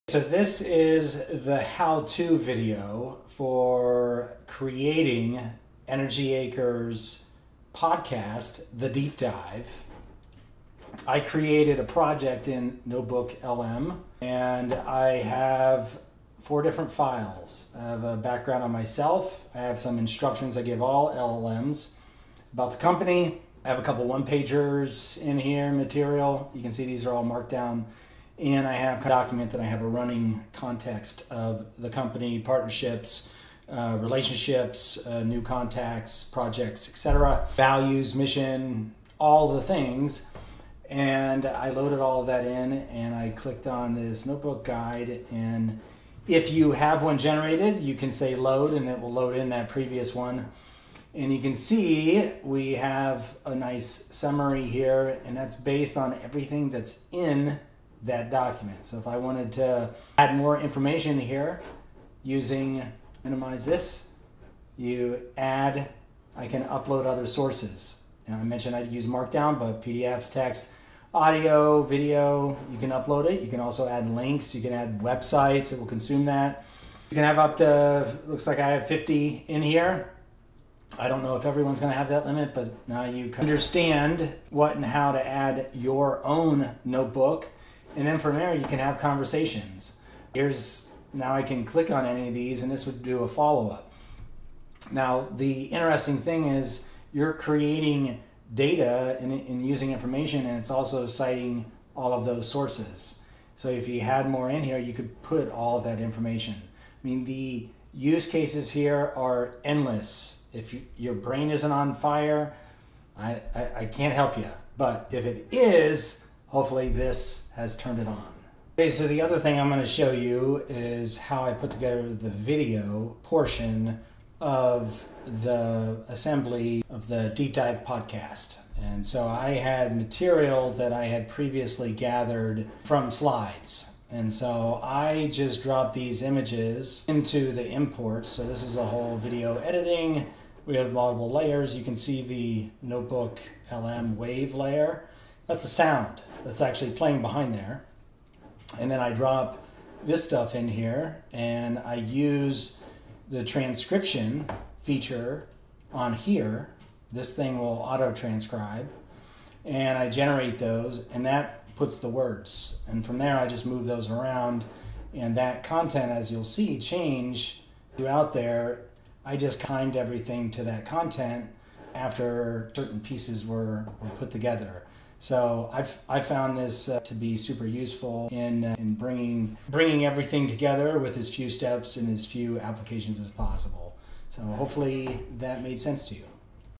Making the Deep Dive - Learn how to use NotebookLM and Build a Podcast
Shawn Cutter showcasing the impressive utility of AI tools for creating the EnergiAcres Deep Dive Podcast:

In this how-to video, Shawn Cutter demonstrates the power and simplicity of using AI to create an engaging podcast. He walks us through his EnergiAcres Deep Dive Podcast project, built using Notebook LM, showcasing its incredible utility in managing vast amounts of data and automating key tasks.

Key Highlights:

 • AI-Powered Podcast Creation: Shawn uses Notebook LM to organize content from multiple files, including markdown documents, PDFs, text, and audio and video files, making the podcast creation process streamlined and efficient.
 • Document Integration & Summarization: Shawn uploads his company’s background, partnerships, mission, and values into the system, generating a clear summary based on all the uploaded materials. He demonstrates how users can continuously add new information to enrich the context.
 • Multimedia Import & Editing: The video shows how easily Shawn integrates slides and images into the podcast, along with layers of sound using Notebook LM’s wave layer feature. He highlights the system’s auto-transcription capabilities, which simplify editing by syncing content with the video timeline.
 • Seamless Workflow: Shawn emphasizes the efficiency of this process, minimizing the number of tools needed and keeping everything organized and easily accessible, even allowing for source citation automation.